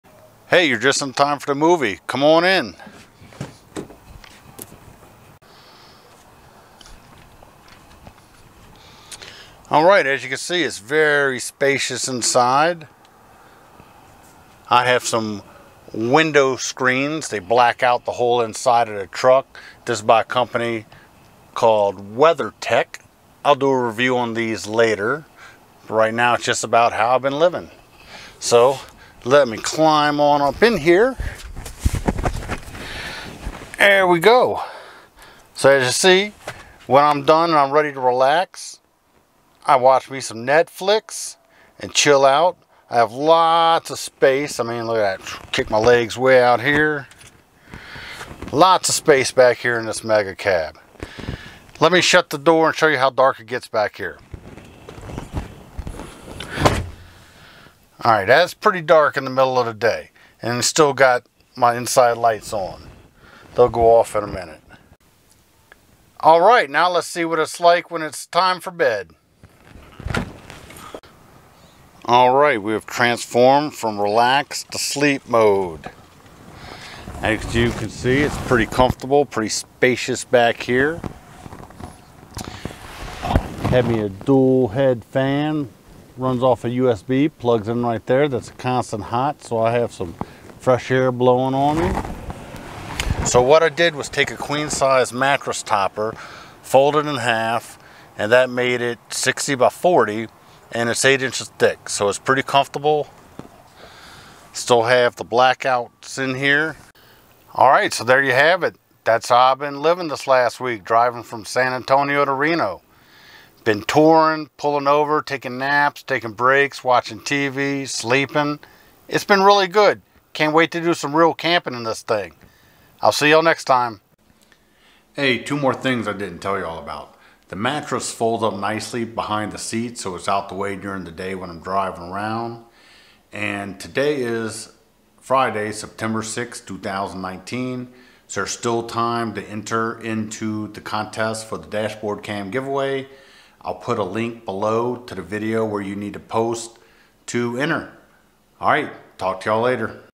How not to spend money on a Hotel while road tripping?
Sleeping in my Ram 2500 Mega Cab while driving across country.